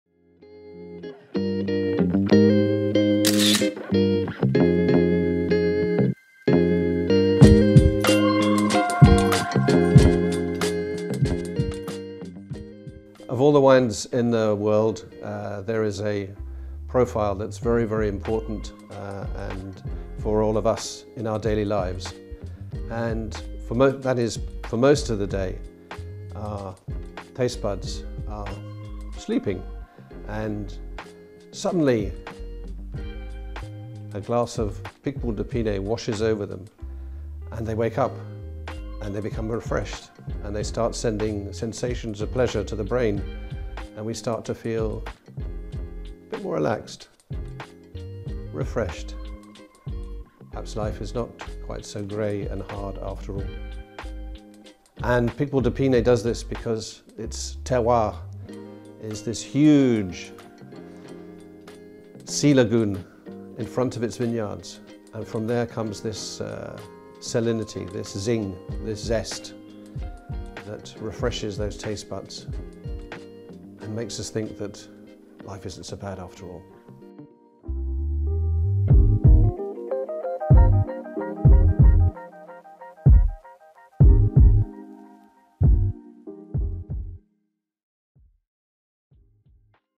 8. Kim Tidy et l'AOP Picpoul de Pinet
Désormais installé en France, Kim Tidy (Arte Potendi) a été directeur d'achats de vins français chez un distributeur britannique. Fin connaisseur des vins de l’appellation, il partage dans sa langue natale l’effet que lui procure la dégustation d'un Picpoul de Pinet.

"Passeurs de goût", la saison 2 de la série 𝘕𝘰𝘴 𝘵𝘦𝘳𝘳𝘦𝘴 𝘮𝘢𝘳𝘪𝘯𝘦𝘴 part à la rencontre des professionnels et des ambassadeurs de l'AOP Picpoul de Pinet.

Conseil Interprofessionnel des Vins du Languedoc
La Région Occitanie / Pyrénées-Méditerranée

Réalisation VITINOVA